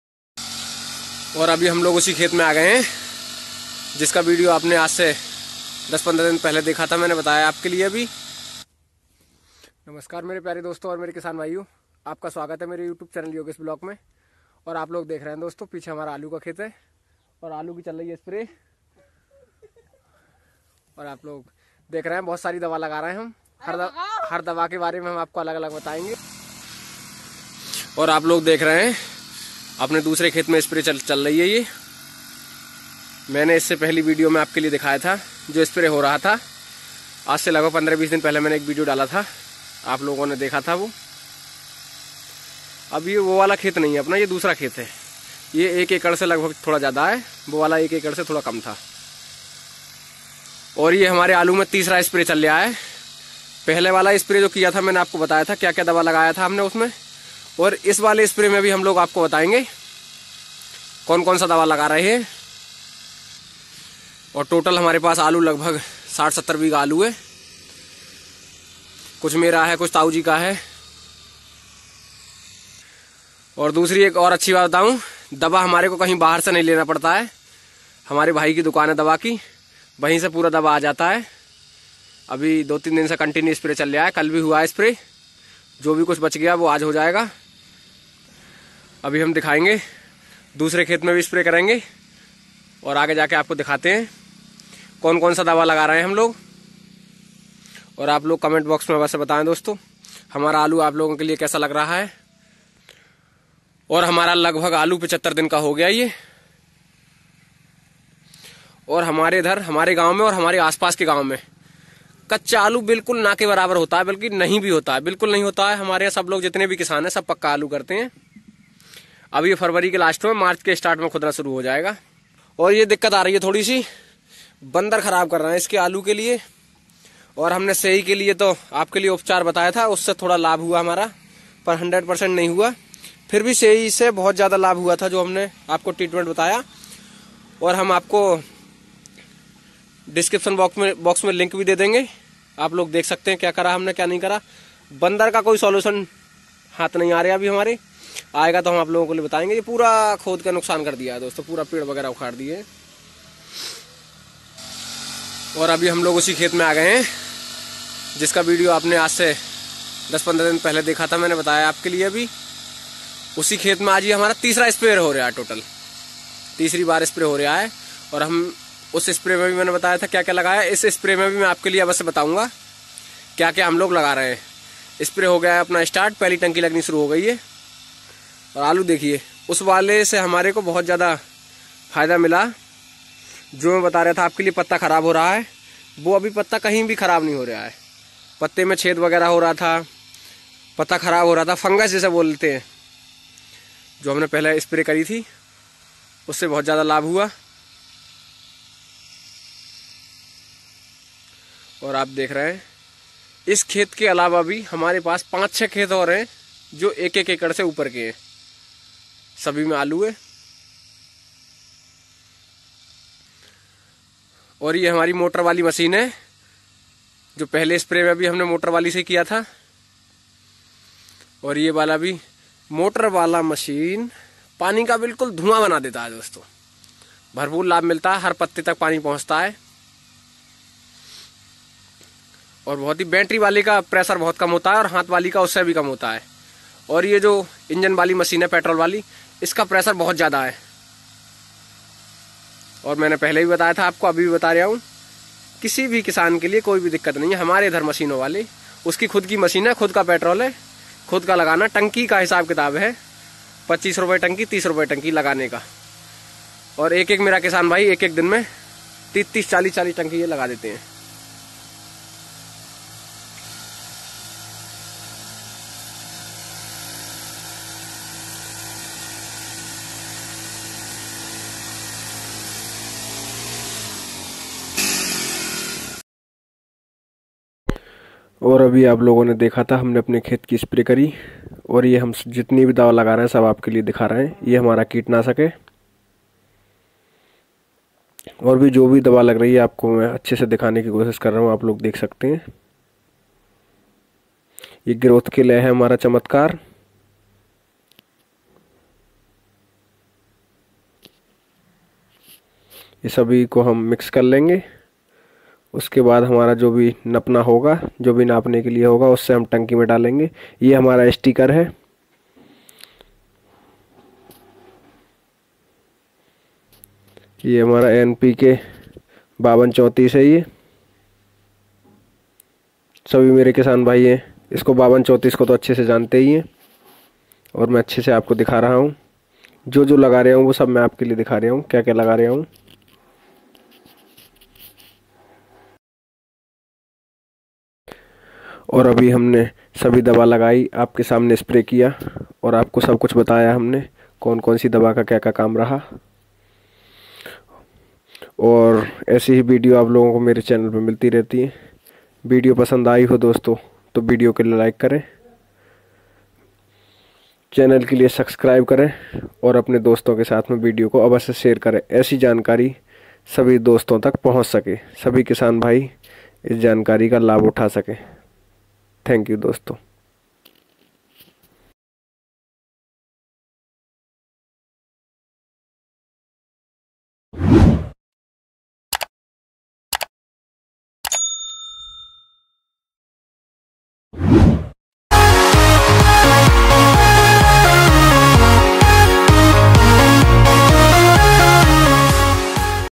आलू की खेती कैसे करें?स्प्रे करने का सही टाइम। ।
आलू की खेती कैसे करें? आलू हो गया 75 दिन का।